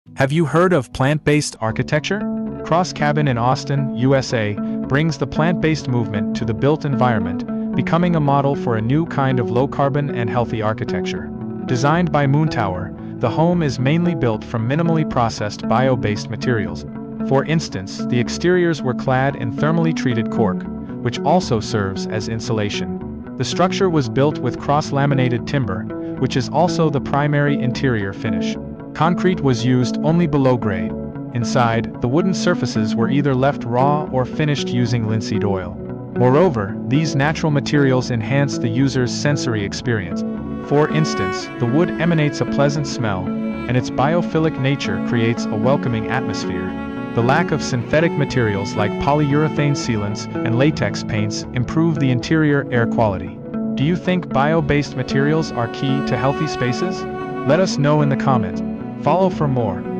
Have you heard of “plant-based” architecture?
Conceived by Moontower, Cross Cabin in Austin, USA, brings the plant-based movement to the built environment.

Made primarily with natural materials like cork and timber, this home explores the potential of bio-based materials in creating a new kind of low-carbon and healthy architecture.

Learn more in the video.

📸: Moontower, Daniel Cavazos, Casey Woods

📹: Passive House Accelerator, Matt Risinger